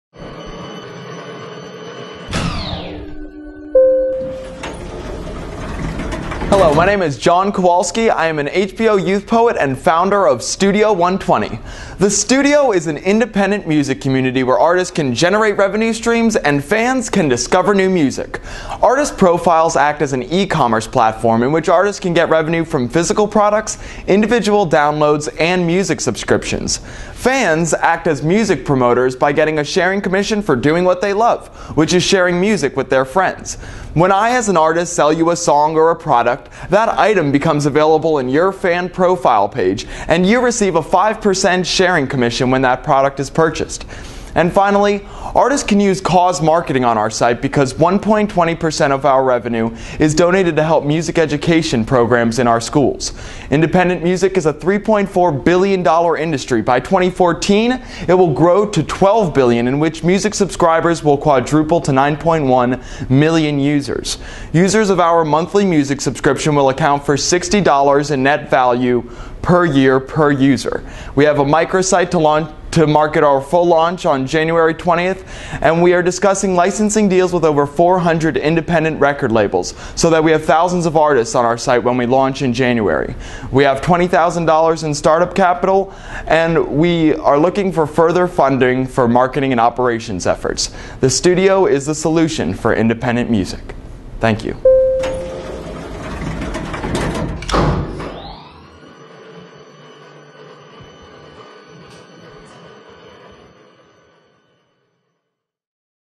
Studio120 (Elevator Pitch)
A finalist in the 2011 Elevator Pitch Contest organized by The Launch Pad at The University of Miami.